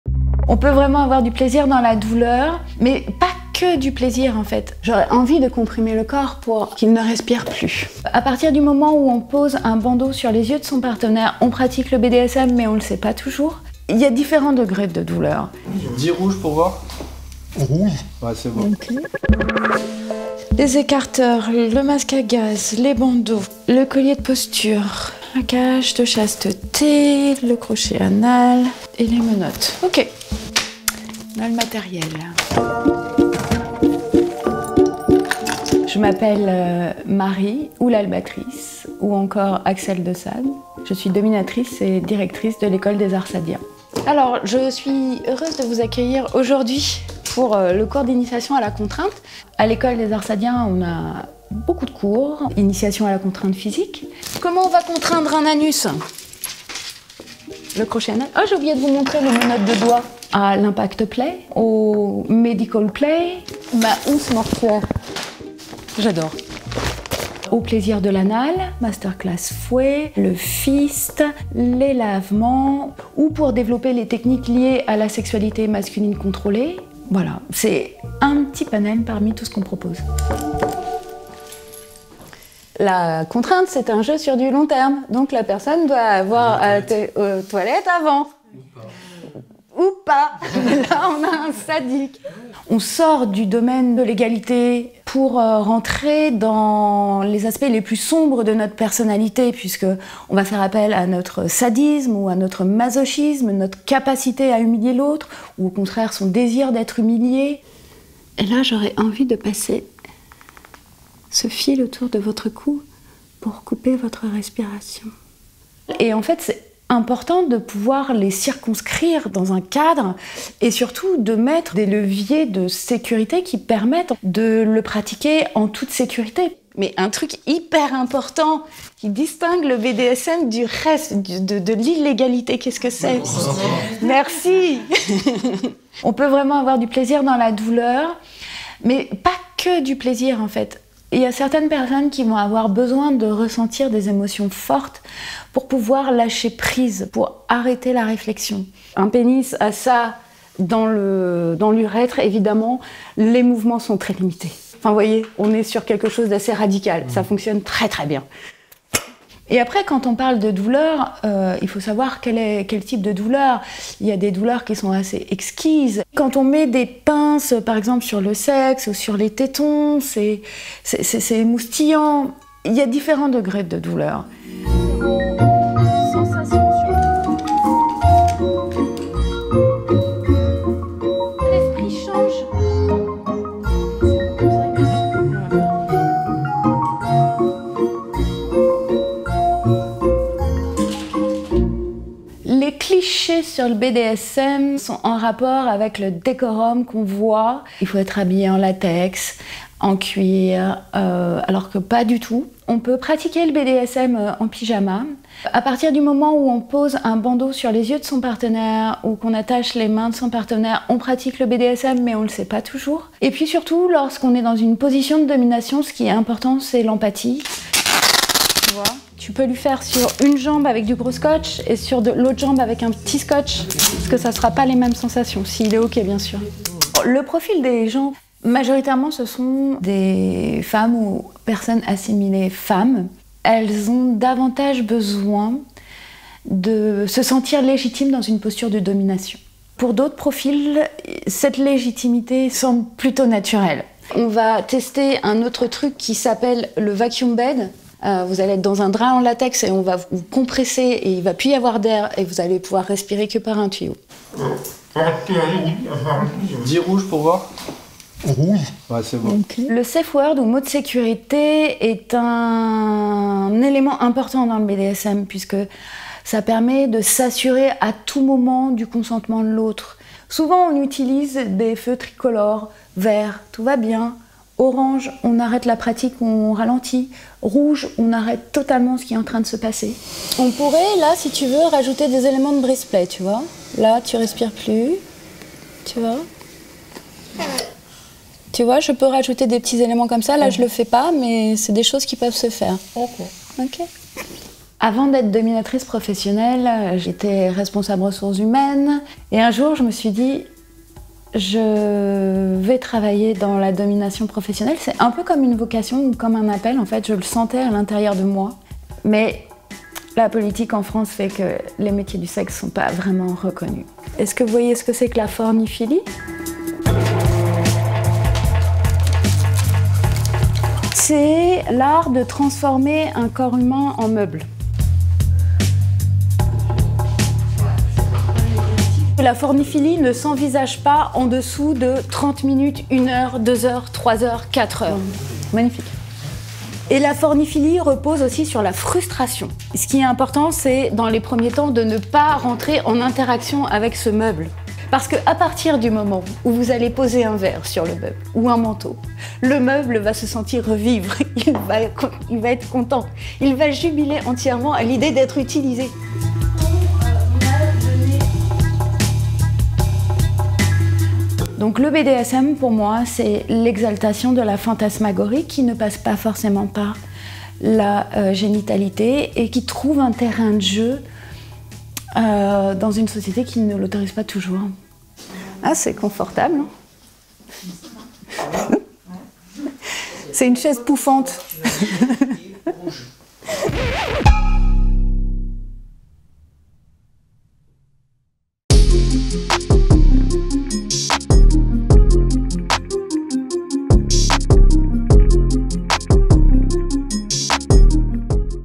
L'école du BDSM : de RH à dominatrice pro, Marie enseigne cette pratique aux débutants | Reportage
À l'école du BDSM, Marie dite aXelle de Sade, enseigne à ses élèves le maniement du fouet, la pénétration anale ou encore le plaisir de la contrainte. On a passé une journée avec cette ancienne DRH devenue dominatrice professionnelle.